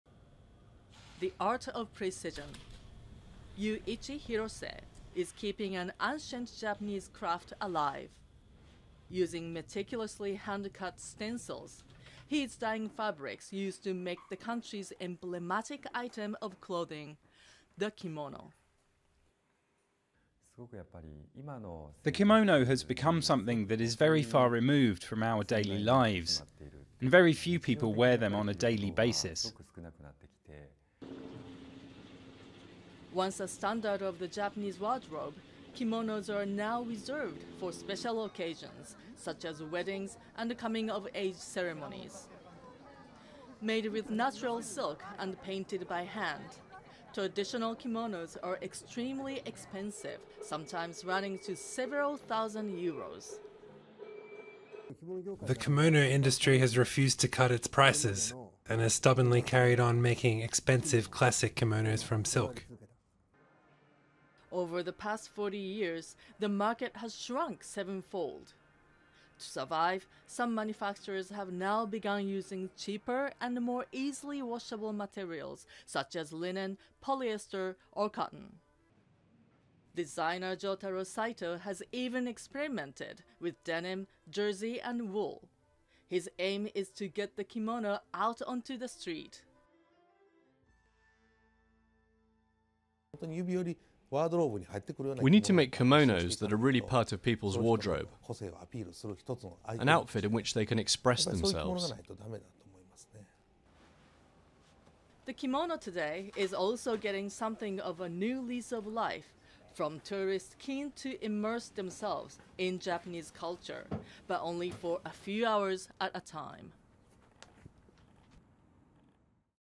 Japanese kimono makers seek to revive declining industry
More videos on Rappler Follow Rappler for the latest news in the Philippines and across the globe. Once a standard of the Japanese wardrobe, the kimono is now often a garment reserved for special occasions, such as weddings and coming-of-age ceremonies, and is mostly worn by women. Full story